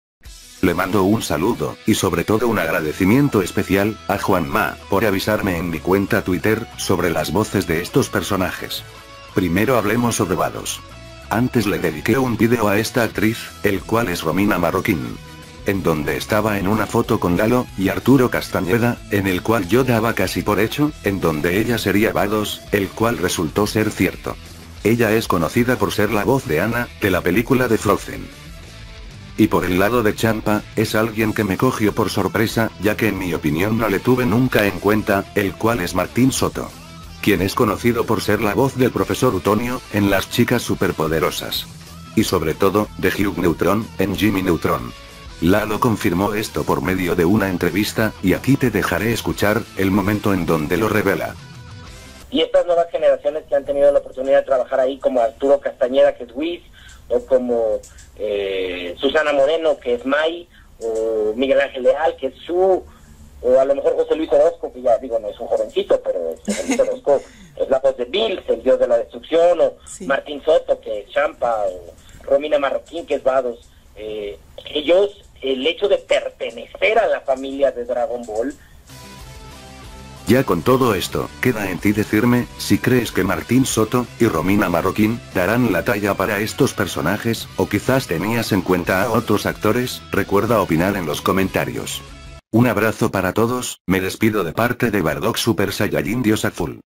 SE CONFIRMA LA VOZ DE CHAMPA Y VADOS PARA EL DOBLAJE LATINO DE DRAGON BALL SUPER | Bardock
VOZ DE CHAMPA Y VADOS SON FINALMENTE CONFIRMADAS.

Mis 3 cuentas Amino:

Anime Amino : pepeanime97

Dragon Ball Amino: Bardock Super Sayayin Dios Azul

Naruto Amino : pepeanime97